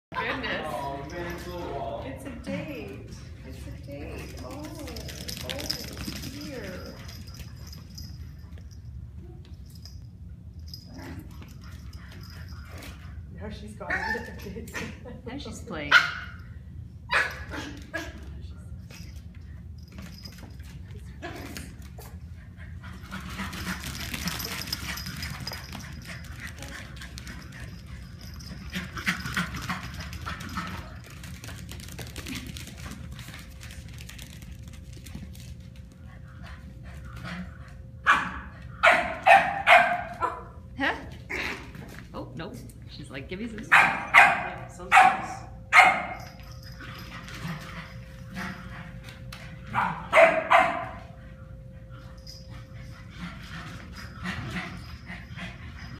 Fitz and Olive in Class 1
A Spice pup and a Jeannie pup in class together.